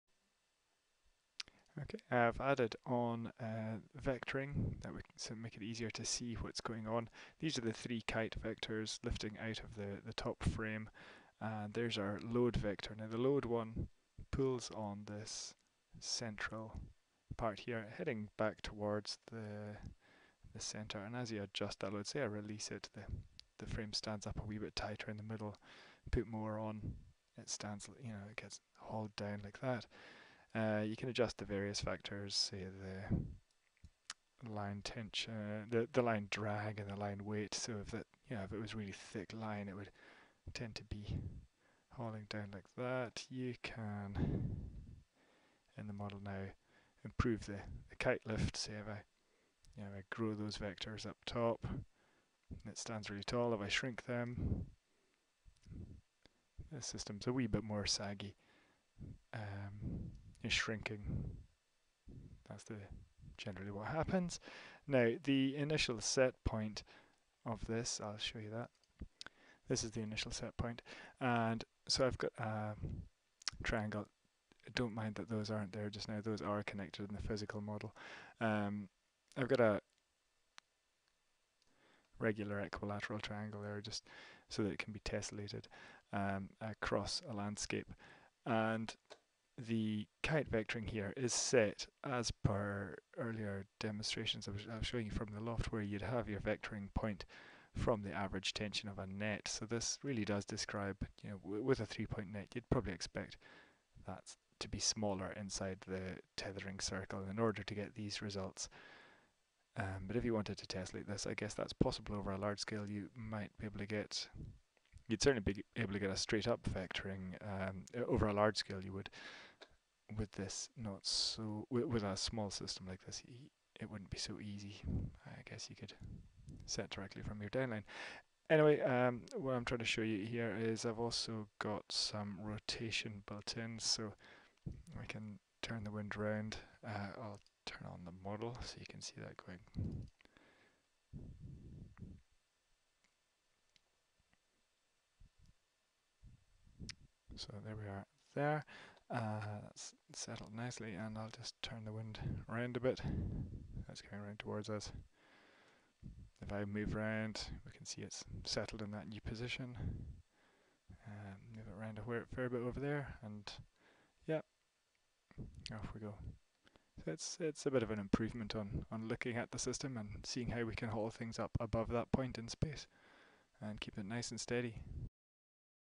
Kite lift frame relative vectoring needs
lifting frame for kite airborne wind energy. The rigging is set under an equilateral triangle so that it can be tessellated.